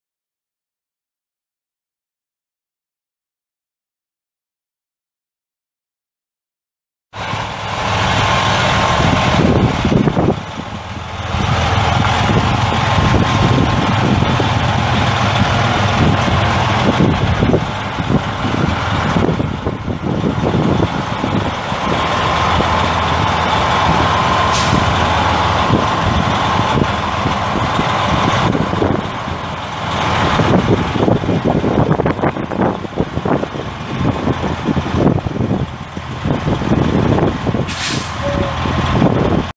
2005 Freightliner Columbia semi truck for sale | no-reserve Internet auction May 18, 2017
2005 Freightliner Columbia semi truck
708,594 miles on odometer
302 HP
Frightliner 10 speed manual transmission
Tandem axle
AC and heat
Tilt
Cruise
AM/FM/CD
Sleeper
Single bed
Storage cabinets
Hanging closet
Exterior visor
Quarter fenders
Mud flaps
Air suspension
Air brakes
295/75R22.5 tires
Iowa title
1FUJA6CK45LV10027
It's located in Hereford, TX, and we're selling it to the highest bidder regardless of price.